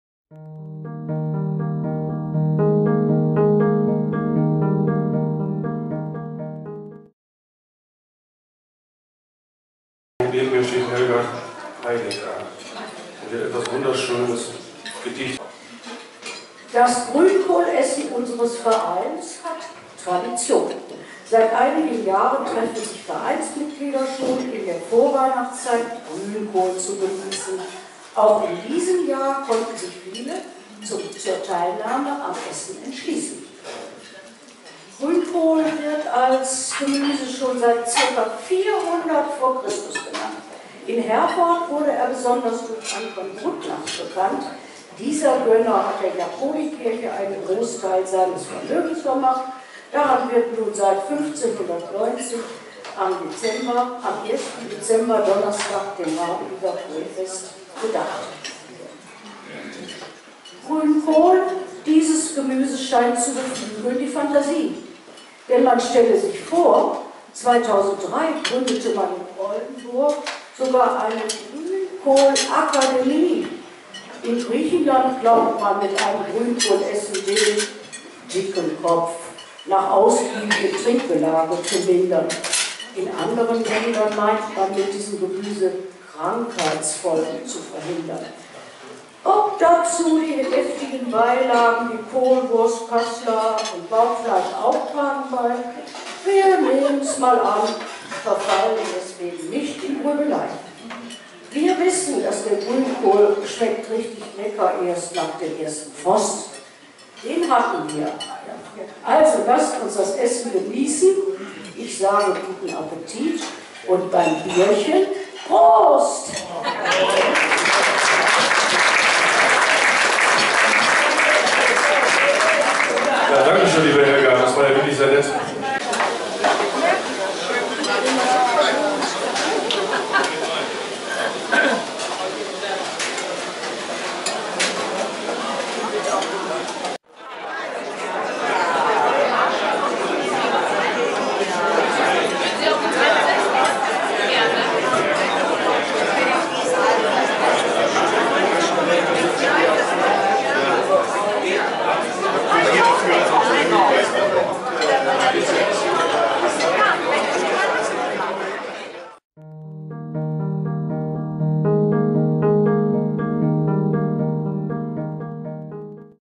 Freundeskreis Herforder Bismarckturm - Grünkohlessen 2014 - 2-4
4. Dezember im Hotel Waldesrand - "Grünkohlgedicht"